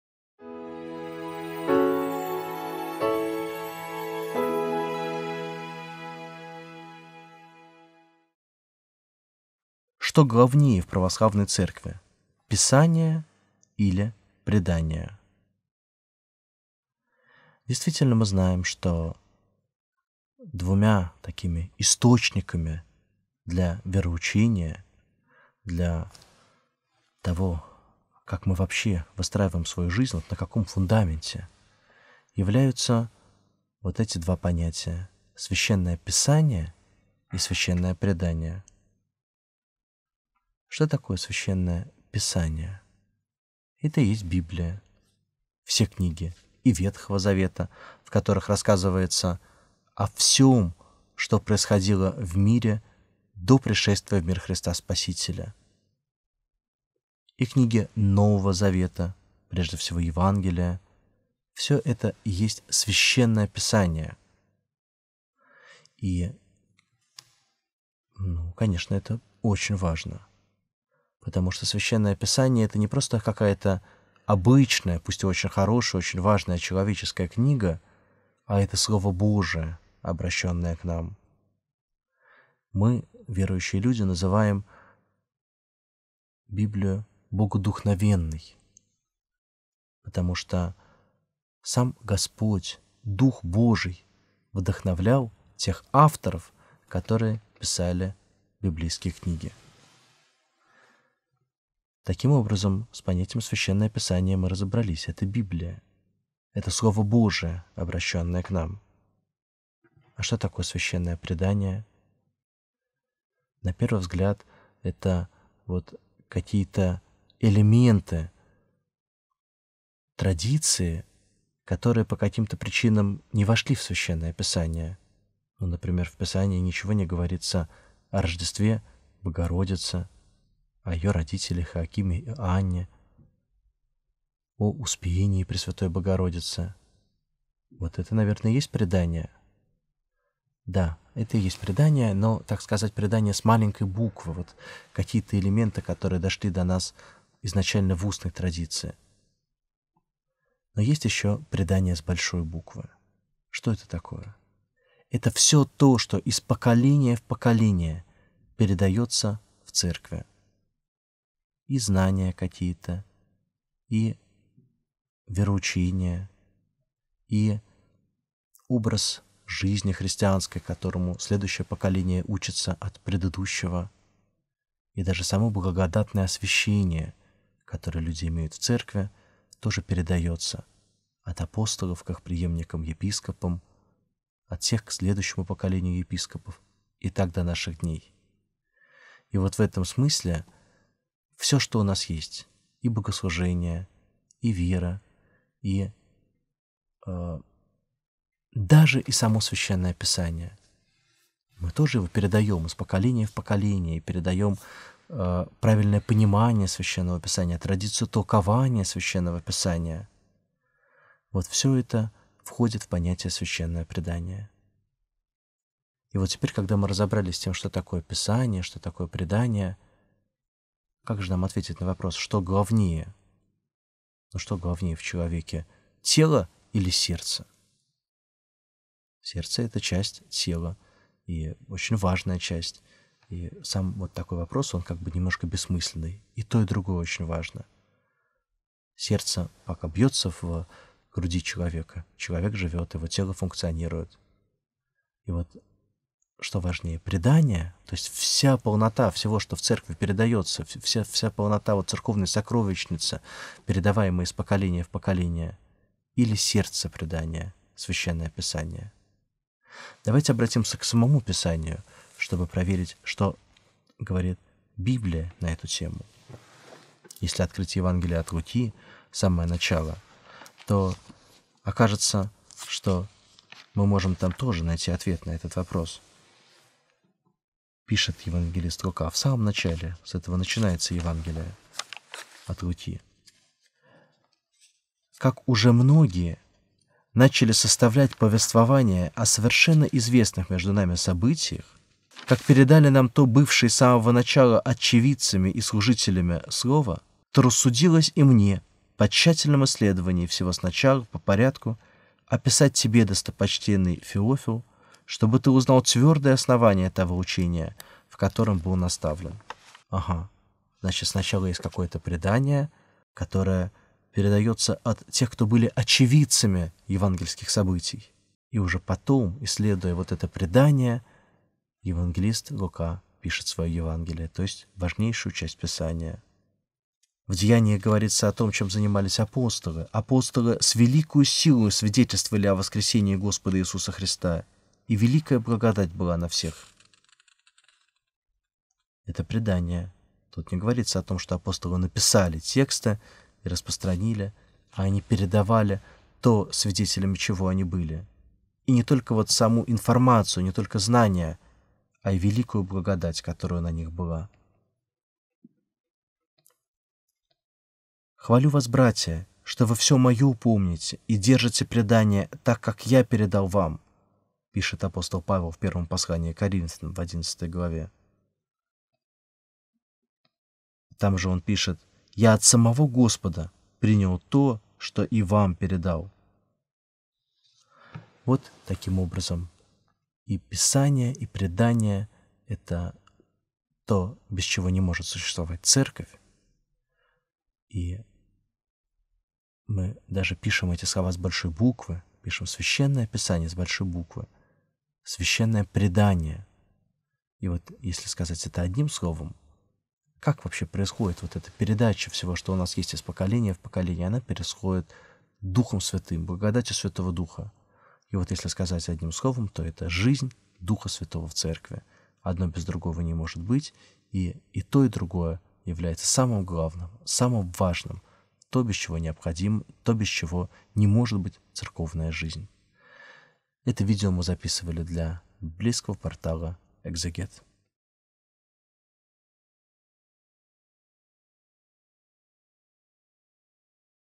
Что главнее : Писание или Предание? Библия отвечает. Священник Григорий Геронимус
В сегодняшнем видео со священником Григорием Геронимусом мы будем говорить о том, что главнее  - Священное Писание или Священное Предание, и коснемся следующих тем:

🔸Фундамент нашей жизни.
🔸Священное Писание.
🔸Мир до пришествия Христа.
🔸Благая весть.
🔸Традиции, которые не вошли в Священный текст.
🔸Правильное понятие Священного Писания.
🔸Что все-таки важнее?
🔸Свидетельство святых апостолов.
🔸Без чего не может существовать церковь?